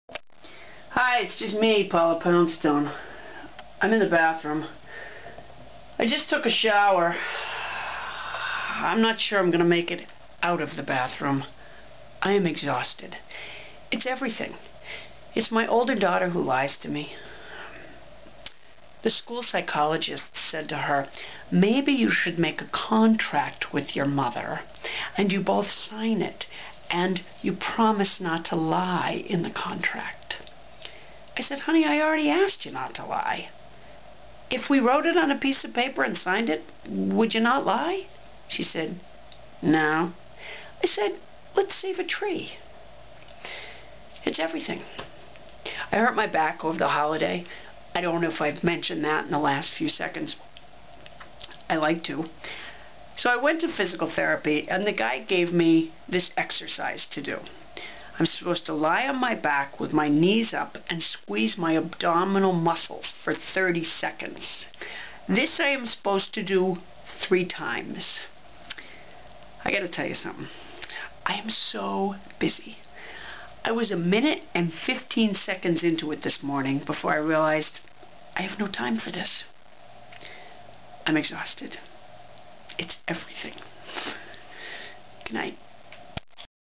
Tired Bathroom
Paula Poundstone is so exhausted, she may not make it out of the bathroom.